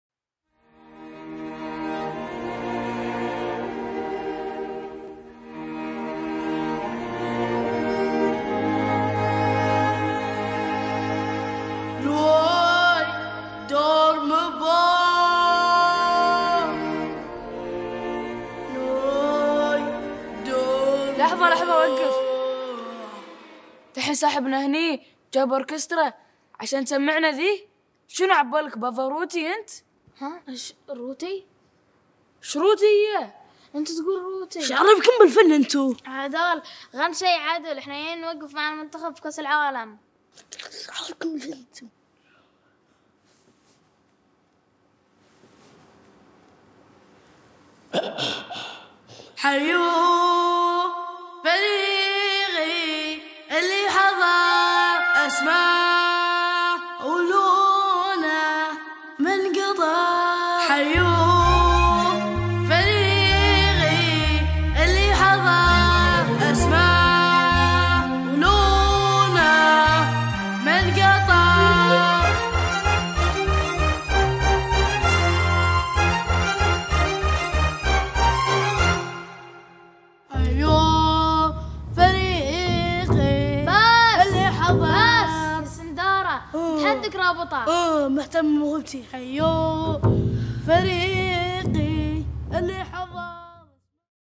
‏عيالنا يشجعون العنابي على طريقتهم الخاصة‬⁩
‏عيالنا يشجعون العنابي على طريقتهم الخاصه 🇶🇦🤩🎻